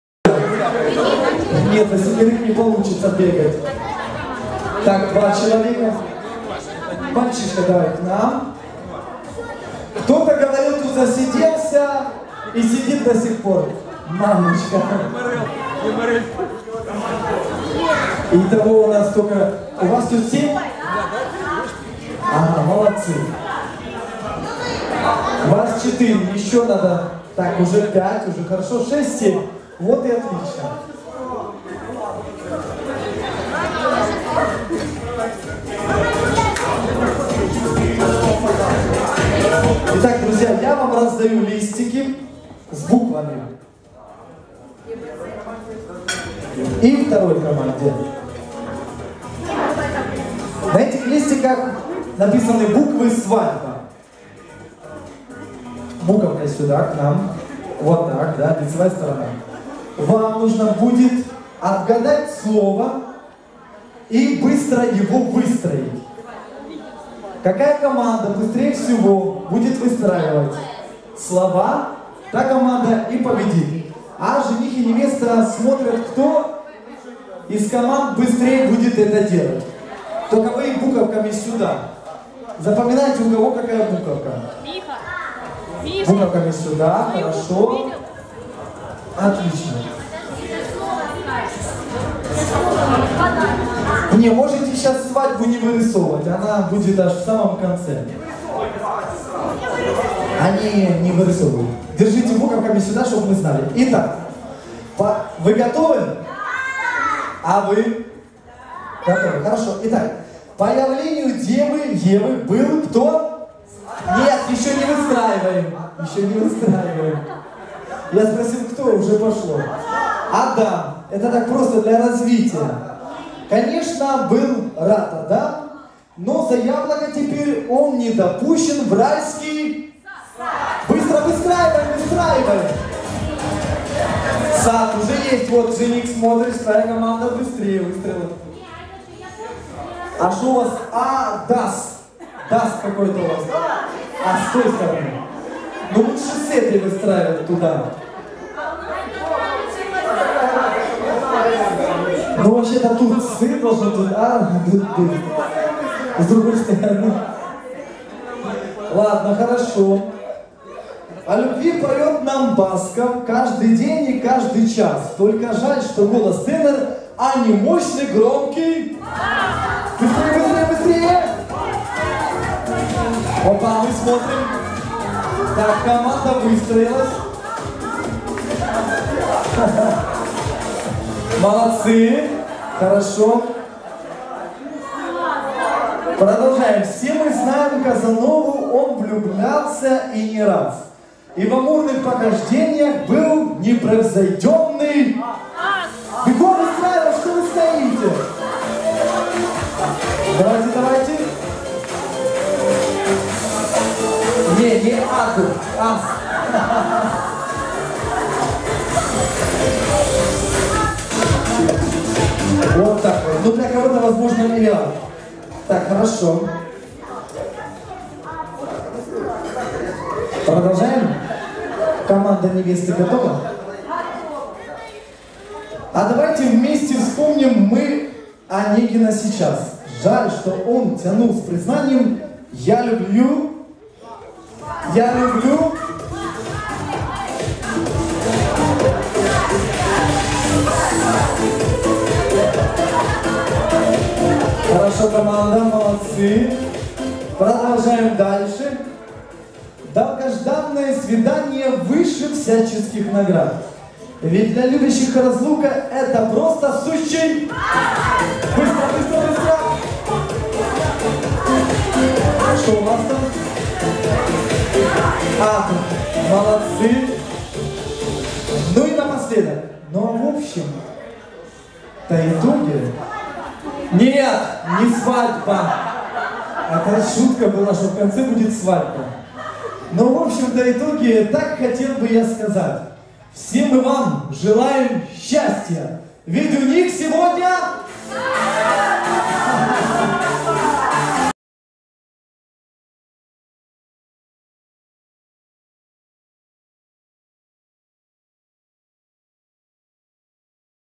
Прикольные и угарные конкурсы и игры на свадьбе! Конкурс с буквами!
Друзья я уже говорил о том что я  веду свадьбы, праздники, и.т.д так вот хочу показать вам одни из самых популярных конкурсов которые подойдут для любой публики на свадьбе!

Ведущий. Шоумен.

Буквенная перестраивалка -это прекрасное развлечение для  студентов и взрослых, солидных  людей

!Для проведения этой игры понадобятся два комплекта букв с фразой, Свадьба
Ведущий читает текст, а участники , выстраивают ответ.
Подсказка — ответ в рифму!

Лучшие приколы, НУ ОЧЕНЬ СМЕШНЫЕ СВАДЕБНЫЕ КОНКУРСЫ, И ЗАЖИГАТЕЛЬНЫЕ ТАНЦЫ В СКОРОМ ВРЕМЕНИ БУДЕТ ТУТ